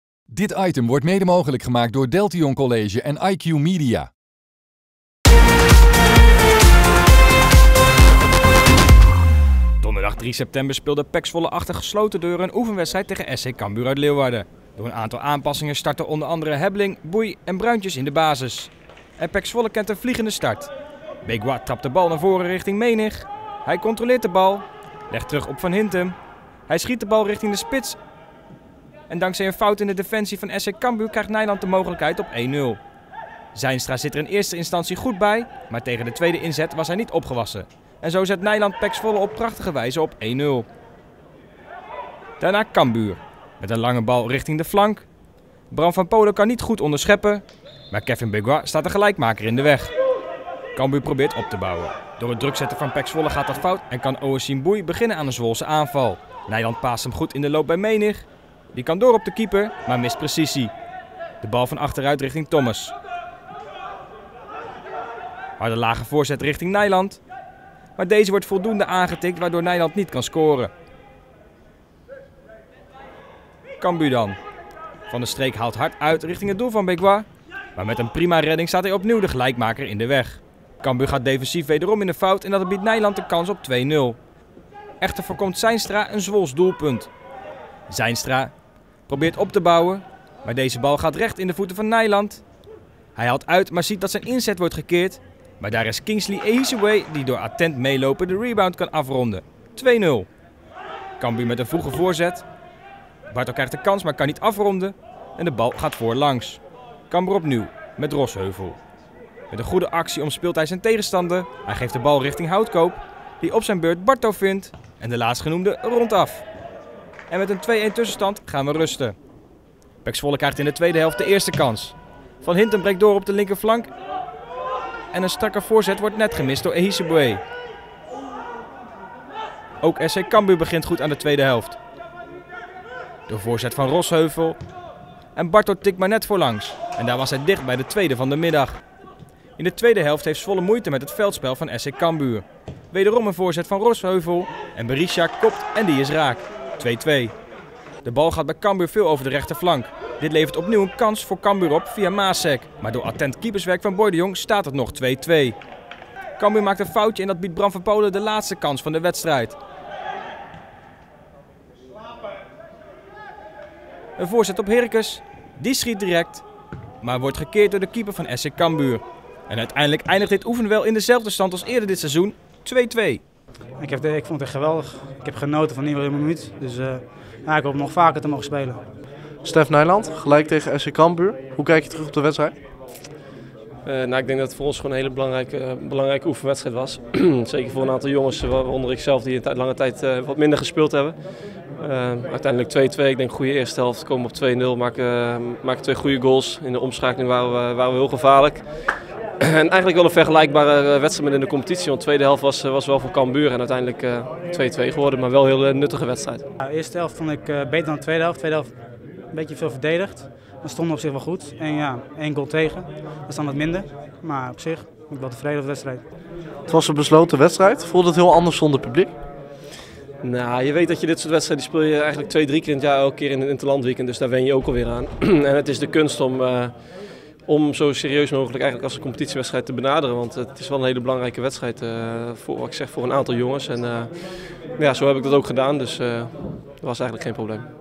Samenvatting oefenduel PEC Zwolle - SC Cambuur
Afgelopen donderdag speelde PEC Zwolle een besloten oefenduel tegen SC Cambuur. In dit duel debuteerden onder andere Ouasim Bouy en Mark Bruintjes. Bekijk hier de samenvatting en de reacties van Stef Nijland en Mark Bruintjes.